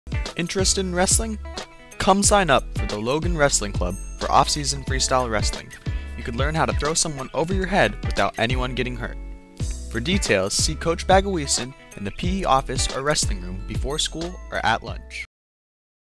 Wrestling Club PSA
A public service announcement produced by Quinn and Jacob for the EMP I course.